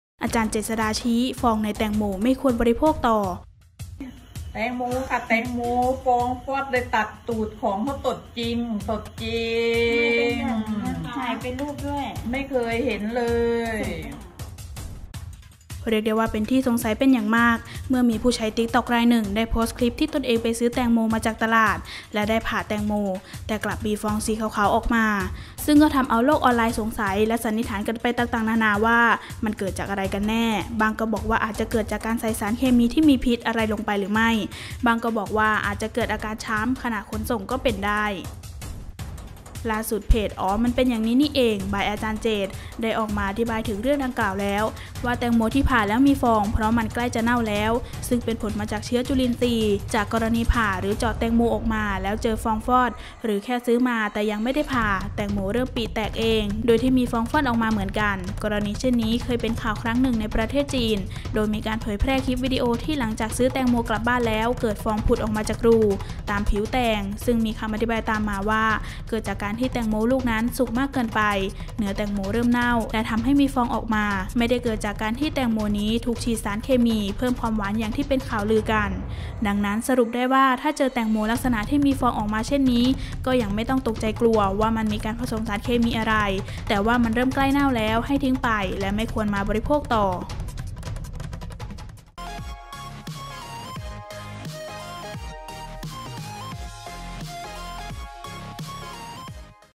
อ.เจษฎาชี้ ฟองในแตงโม ไม่ควรบริโภคต่อ
เรียกได้ว่าเป็นที่สงสัยเป็นอย่างมากเมื่อมีผู้ใช้tiktokรายหนึ่งได้โพสต์คลิปที่ตนเองไปซื้อแตงโมมาจากตลาดและได้ผ่าแตงโมแต่กลับมีฟองสีขาวๆออกมา ซึ่งก็ทำเอาโลกออนไลน์สงสัยและสันนิษฐานกันไปต่างๆนาๆว่ามันเกิดจากอะไรกันแน่ บ้างก็บอกว่าอาจจะเกิดจากการใส่สารเคมีที่มีพิษอะไรลงไปหรือไม่ บ้างก็บอกอาจจะเกิดจากอาการช้ำขณะขนส่งก็เป็นได้
     ล่าสุดเพจ อ๋อ มันเป็นอย่างนี้นี่เอง by อาจารย์เจษฎ์ ได้ออกมาอธิบายถึงเรื่องดังกล่าวแล้วว่า แตงโม ที่ผ่าแล้วมีฟอง เพราะมันใกล้จะเน่าแล้ว ซึ่งเป็นผลจากเชื้อจุลินทรีย์
     กรณีผ่าหรือเจาะแตงโมออกมาแล้วเจอฟองฟอด หรือแค่ซื้อมา ยังไม่ได้ผ่า แตงโมเริ่มปริแตกเองโดยที่มีฟองฟอดออกมาเหมือนกัน กรณีเช่นนี้ เคยเป็นข่าวครั้งหนึ่งในประเทศจีน โดยมีการเผยแพร่คลิปวิดีโอที่หลังจากซื้อแตงโมกลับบ้านแล้ว เกิดฟองผุดออกมาจากรู ตามผิวแตง ซึ่งมีคำอธิบายตามมาว่า เกิดจากการที่แตงโมลูกนั้นสุกมากเกินไป เนื้อแตงโมเริ่มเน่า และทำให้มีฟองออกมา ไม่ได้เกิดจากการที่แตงโมนี้ถูกฉีดสารเคมีเพิ่มความหวานอย่างที่เป็นข่าวลือกัน
     ดังนั้น สรุปคือ ถ้าเจอแตงโมลักษณะที่มีฟองออกมาเช่นนี้ ก็ยังไม่ต้องตกใจกลัวว่ามันมีการผสมสารเคมีอะไร แต่ว่ามันเริ่มใกล้เน่าแล้ว ให้ทิ้งไปและไม่ควรมาบริโภคต่อ